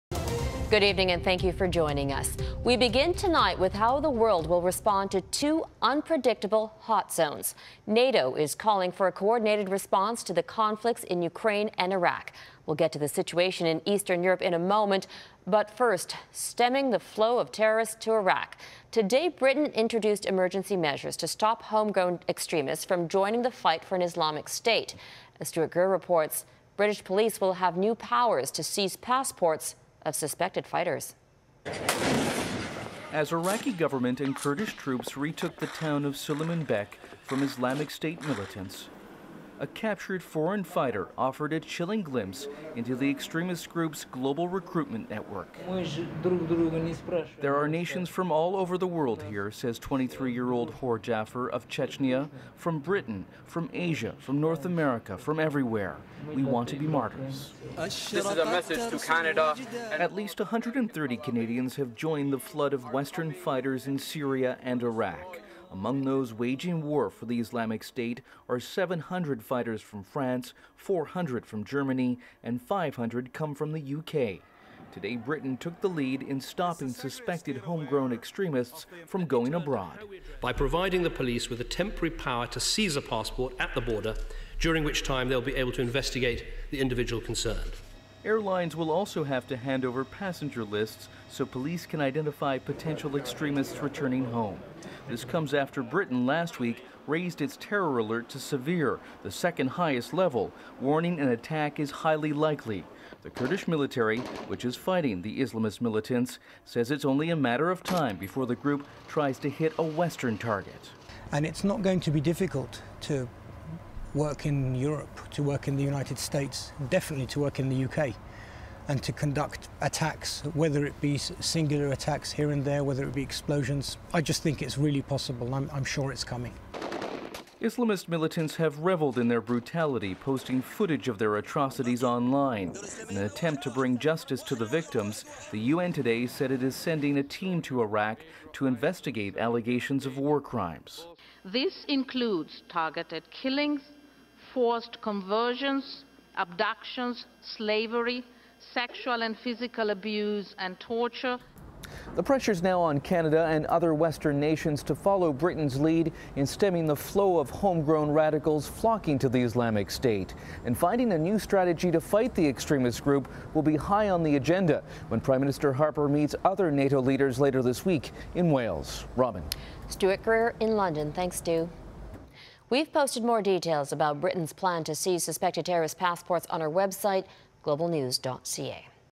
Britain vows to seize passports of potential fighters
Mon, Sep 1: Britain introduced emergency measures to stop homegrown extremists from joining the fight for an Islamic state. As Stuart Greer reports, British police will have new powers to seize passports of suspected fighters.